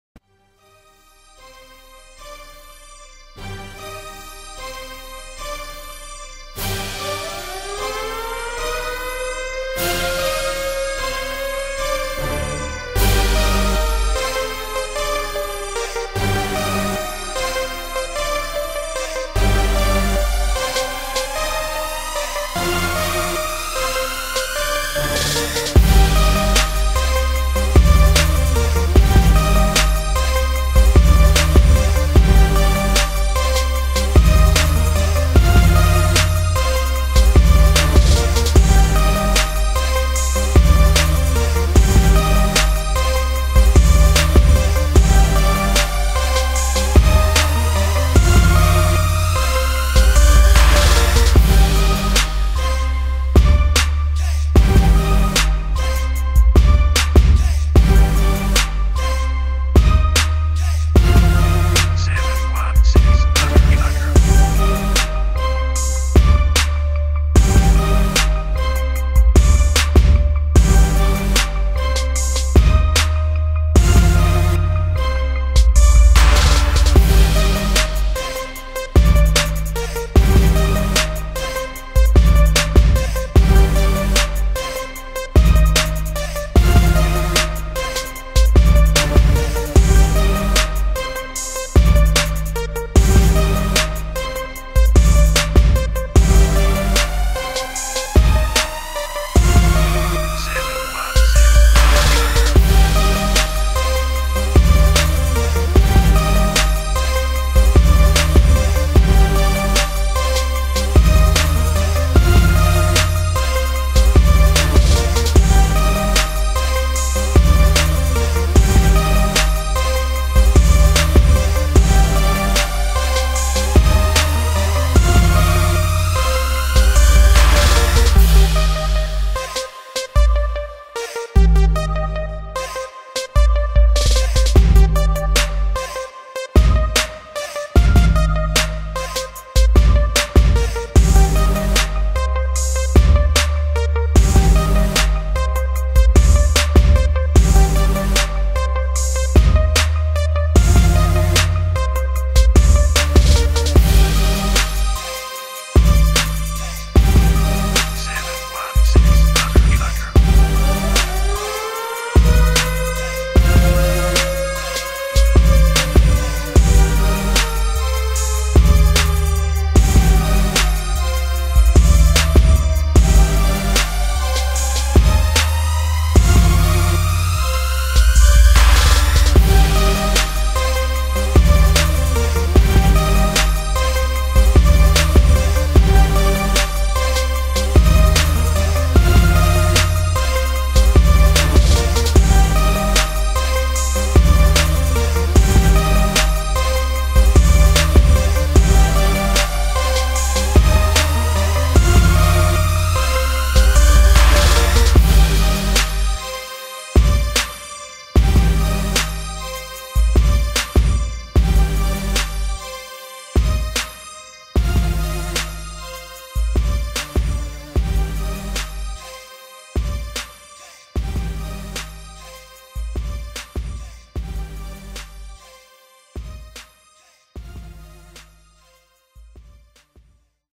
Dark future synth beat
New beat I made in FL Studio 10.

ReverbNation page! All beats are available for $1.99 each.